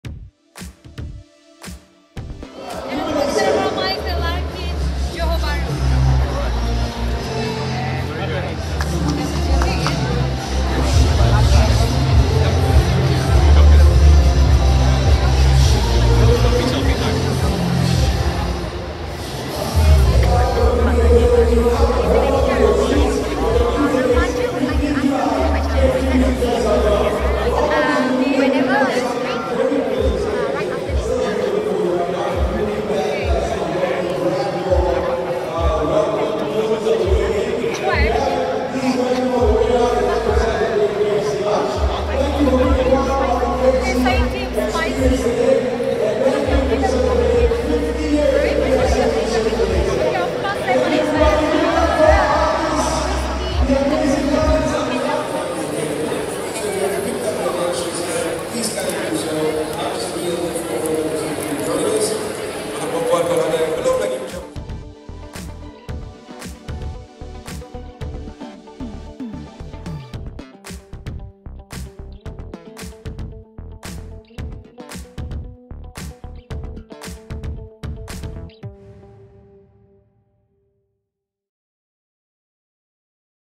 Siti Nurhaliza Ajak Peminat ke Kepci Music Fest di Arena Larkin
Sempena ulangtahun ke-50 KFC Malaysia yang sedap sehingga menjilat jari, korang berpeluang untuk memenangi tiket Kepci Music Fest secara PERCUMA! Bukan selalu korang dapat enjoy layan muzik sampai puas! Berbeza dengan pesta muzik yang lain, Kepci Music Fest akan dilakukan di 3 lokasi yang berbeza. Lokasi berbeza, line up pun mestilah berbeza-beza!Haa, jangan risau! Meh kami kongsi 3 lokasi Kepci Music Fest dan line up para artis yang akan perform di Kepci Music Fest! At least boleh lah korang start hafal lirik lagu diorang! Lokasi Pertama: Stadium Malawati, Shah Alam (26 Ogos 2023) Lokasi Kedua: Kadazan Dusun Cultural Association (KDCA), Sabah (6 Oktober 2023) Lokasi Ketiga: Arena Larkin, Johor Bahru (21 Oktober 2023) *Lokasi mungkin akan berubah